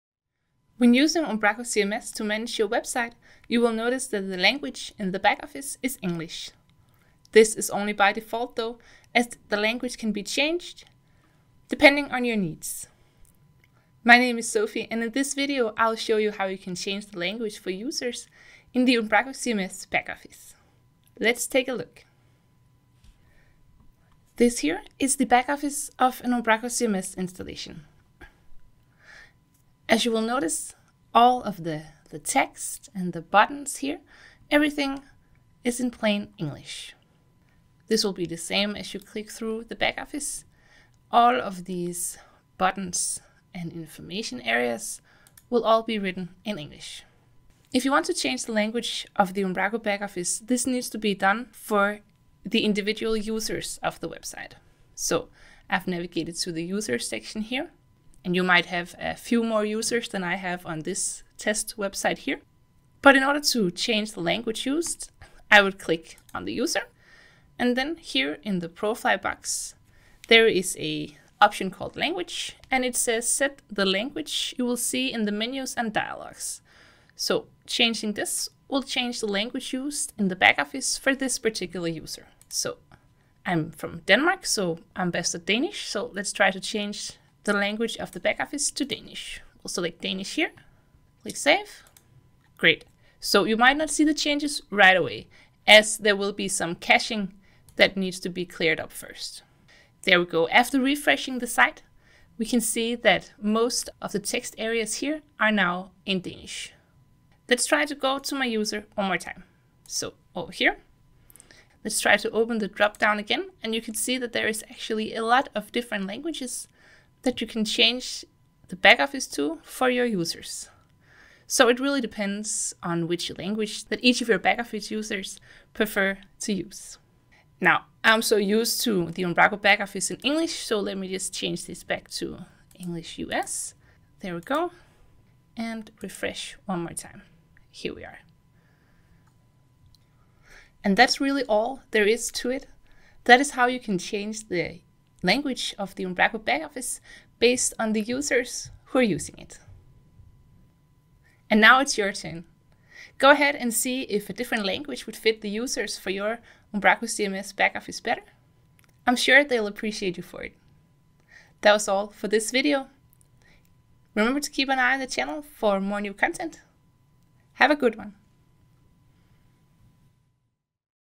Umbraco Basics: Changing the backoffice language
The language used in the Umbraco CMS backoffice is English by default. This can be changed on a user basis, depending on your content editor's preferred language.